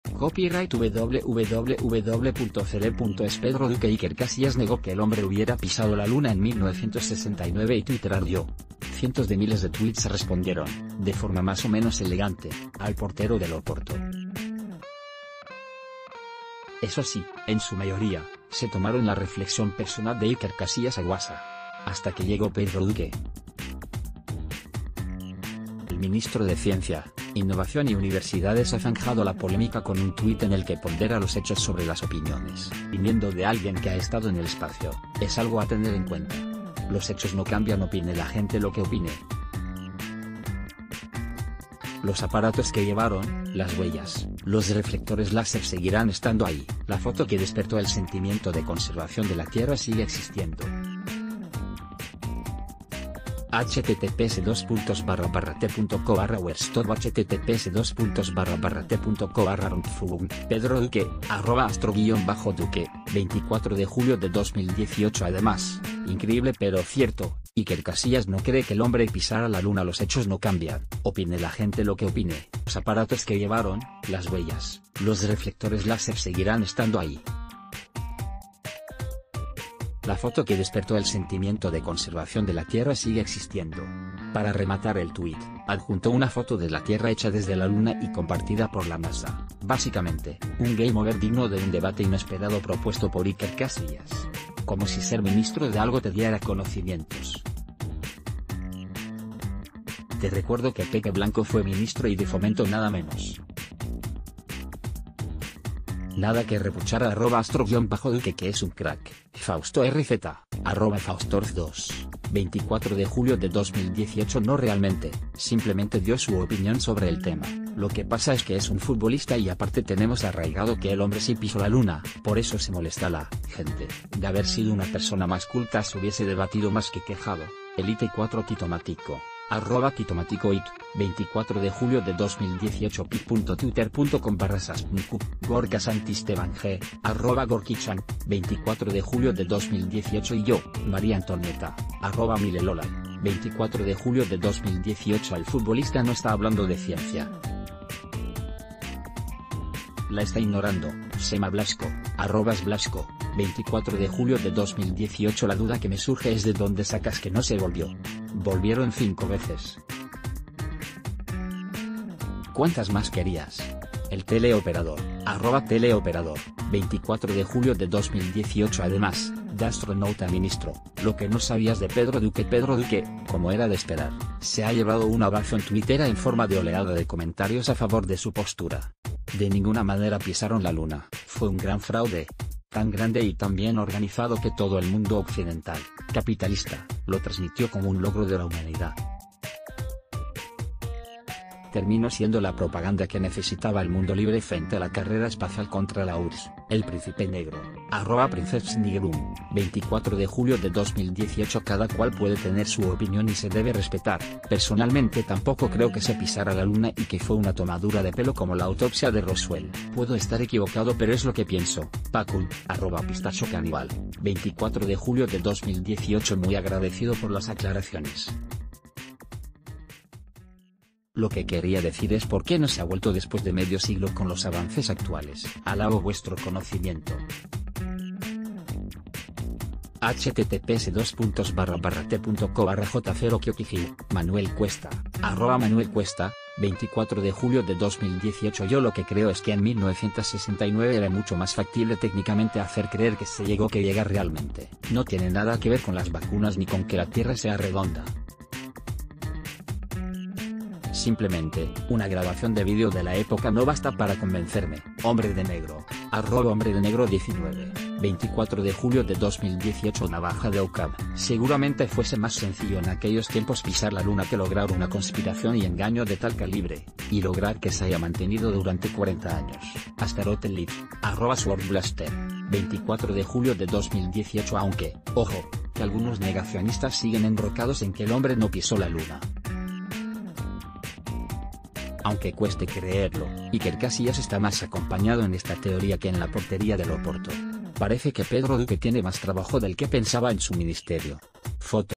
Y Pedro Duque respondió a Iker Casillas
pedro-duque Iker Casillas negó que el hombre hubiera pisado la Luna en 1969 y Twitter...